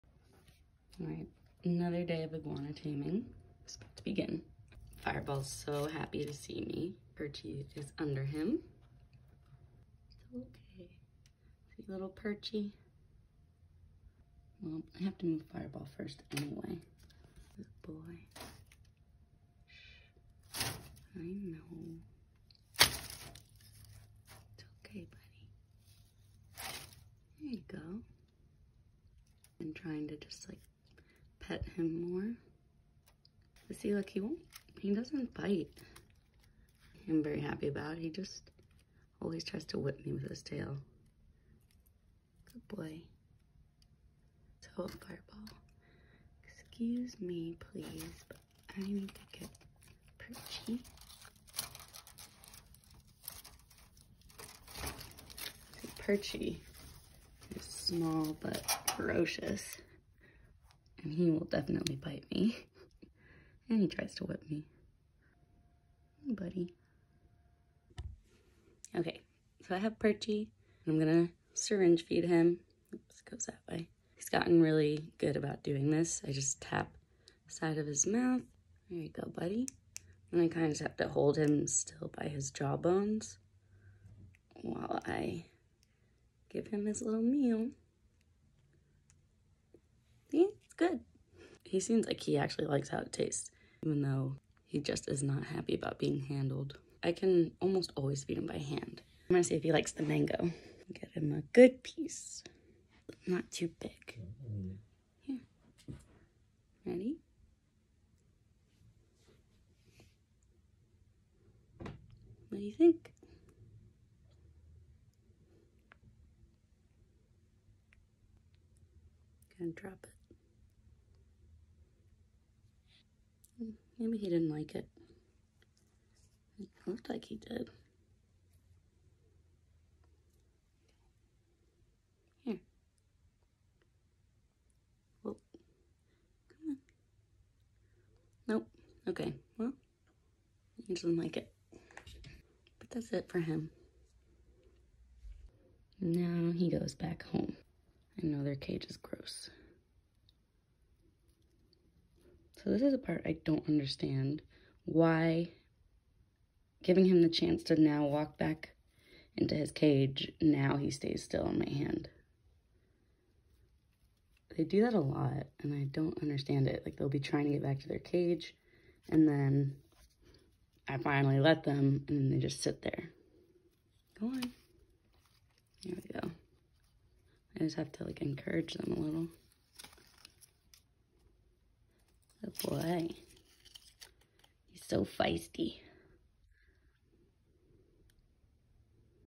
Feeding my sickly green iguana
Here I am feeding Perchy, one of my green iguanas, his Emeraid via syringe which he needs to be fed daily. Perchy is an odd case. In all aspects of his life, he seems like a perfectly normal green iguana; he's feisty, bites, whips with his tail, eats, sleeps, poops, climbs - all of the iguana things!

He seems to be pretty satisfied with life despite the fact that he is basically a living skeleton. I have consulted with an exotic animal veterinarian in my area over the last few months, and after trying different options, we still really don't know why he has failed to grow in the 2 years I've owned him. But we decided that so long as he seems to be  enjoying life and keeps his "will to live" so to speak, I am happy to give him the extra care he needs! :)